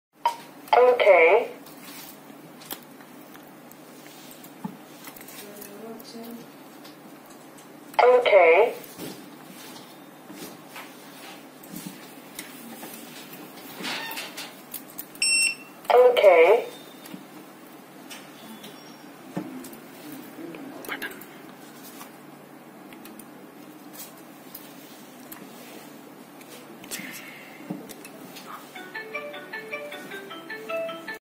TimeWatch - BIO-27 with Exit Readers(Time & Attendance Terminal)
TW-BIO-27 is Color Screen Fingerprint Time & Attendance Terminal Support up to 1,000 Fingerprint/Card / Password its adopts Bio-ID (Fingerprint) collector for Excellent Recognition and Wi-Fi for convenient communication . User can mange data via TCP/IP and USB Host port for data download / upload to avoid the risk of accidental deletion optional could based solution for Remote Location.

For More Information / Details on Products & Software

Take One Step Ahead and Meet with Better Technologies